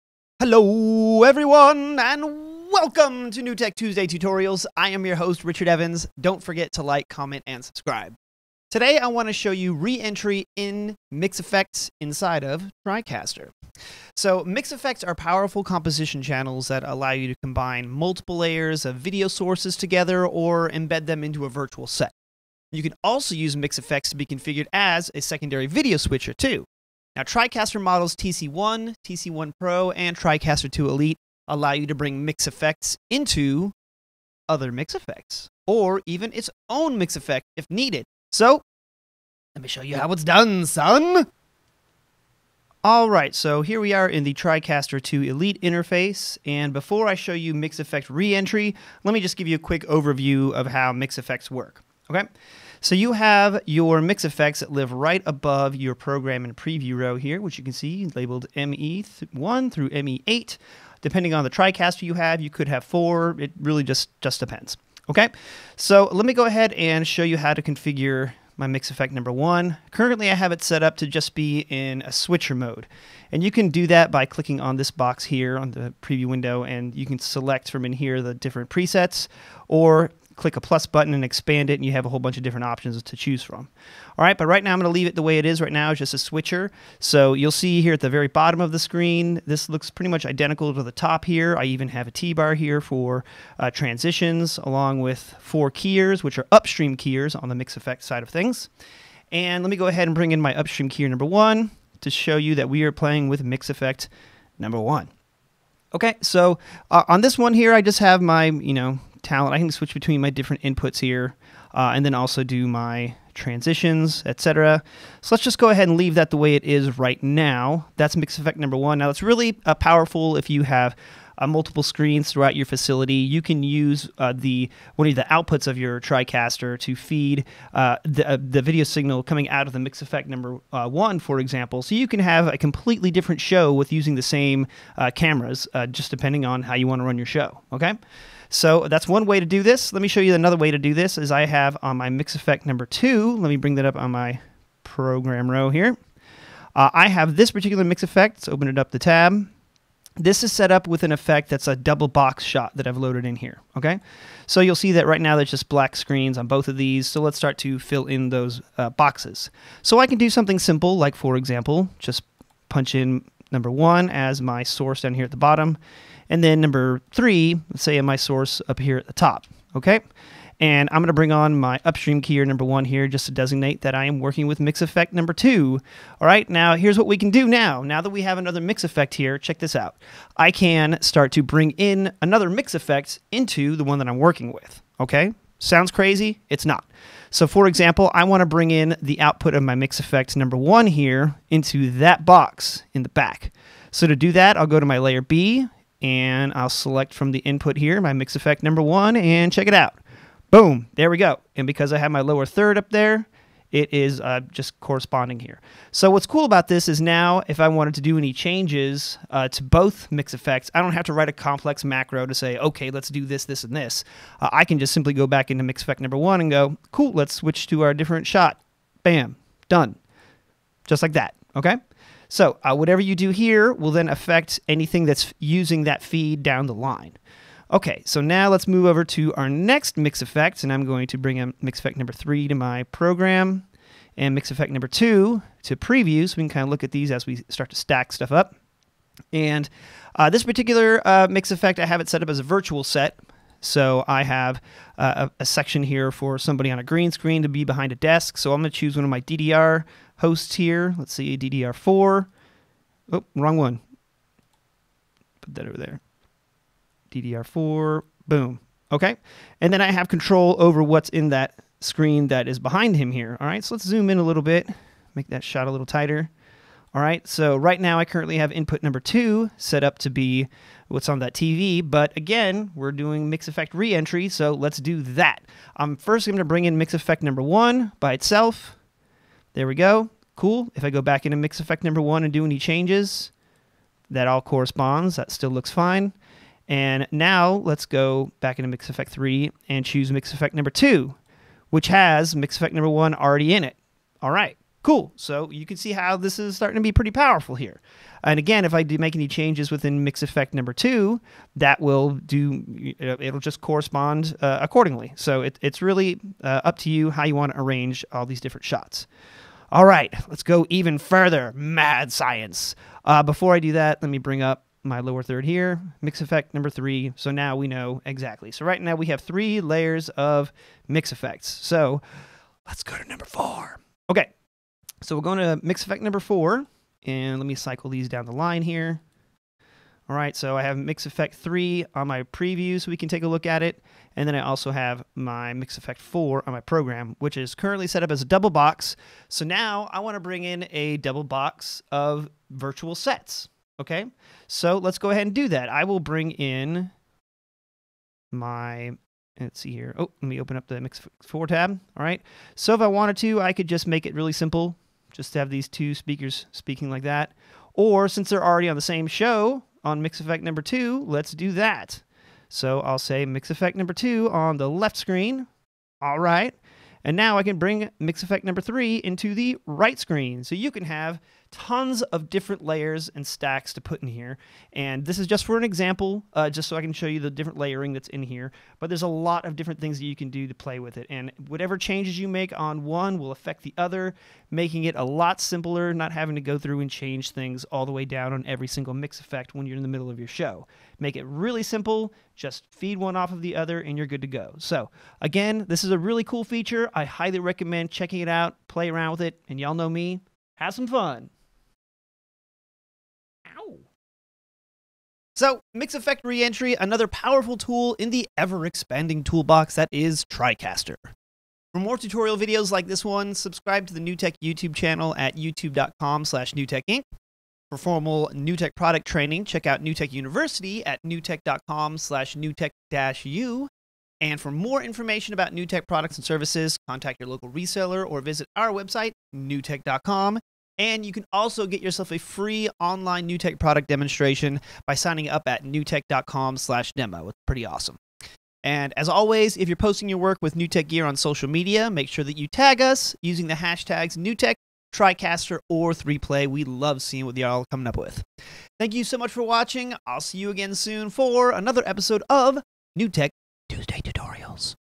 Tuesday Tutorials : Mix Effect ReEntry
Mix Effects are powerful multi layered composition channels allowing you to mix several sources together or embed them into a virtual set.  You can also use each super source or mix effect to mix with other mix effects for picture in picture or other compositions.  In this video we will demonstrate how to re-enter mix effects into other mix effects expanding your TriCaster capabilities.